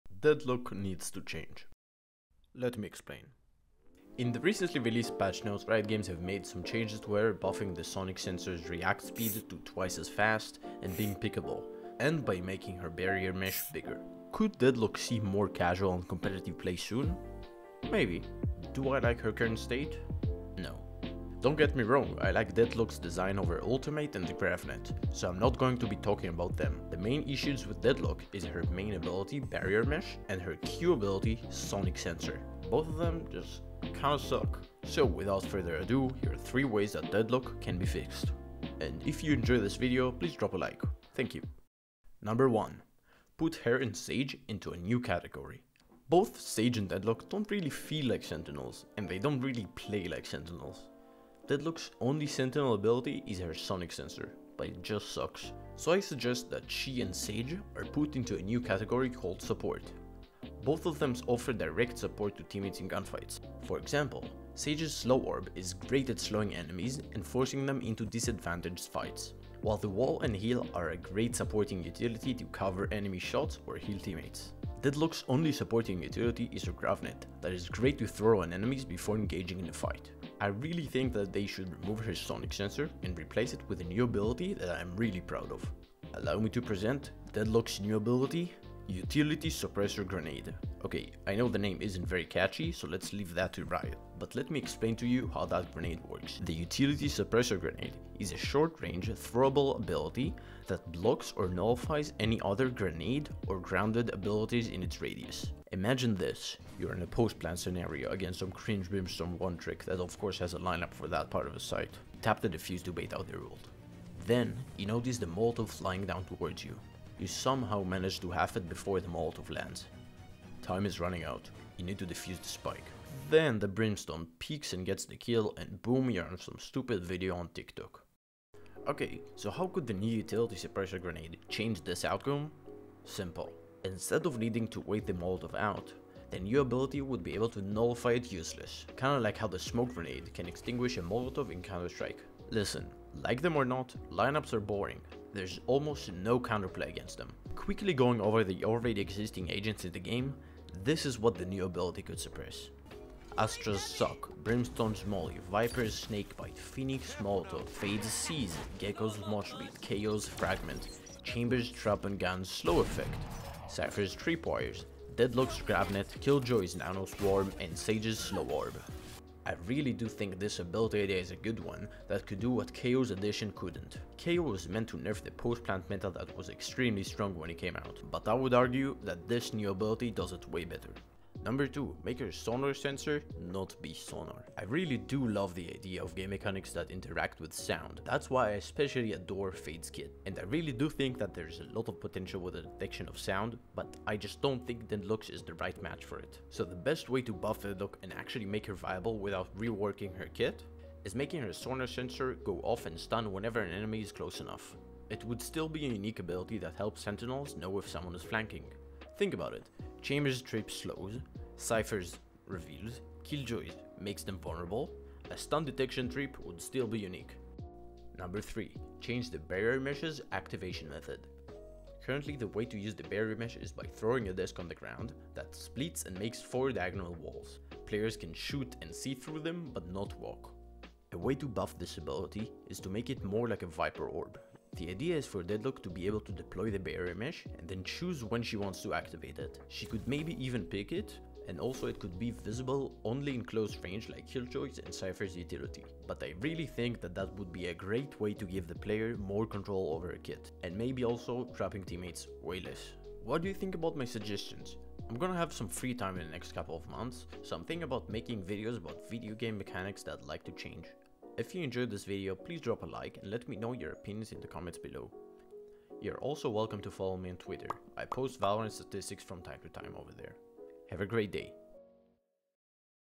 These Changes Could FIX Deadlock
In this video I attempt to give 3 ideas on how to fix deadlock from valorant